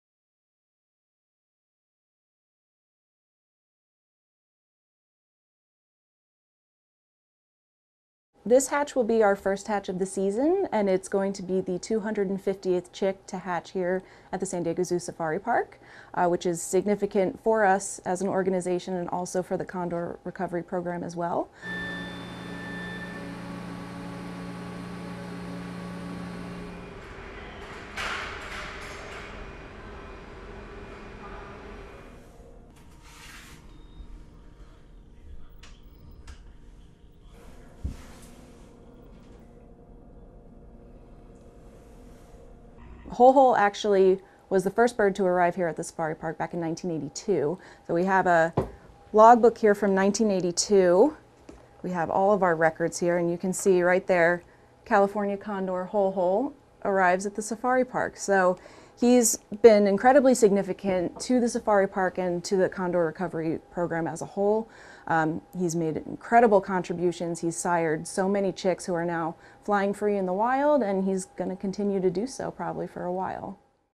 San Diego Zoo Safari Park welcomes 250th California condor chick
San Diego Zoo Wildlife Alliance has welcomed the 250th California condor to hatch at the San Diego Zoo Safari Park. The chick’s parents are Xol-Xol and Mexwe, the first California condor brought into human care under the California Condor Recovery Program in 1982.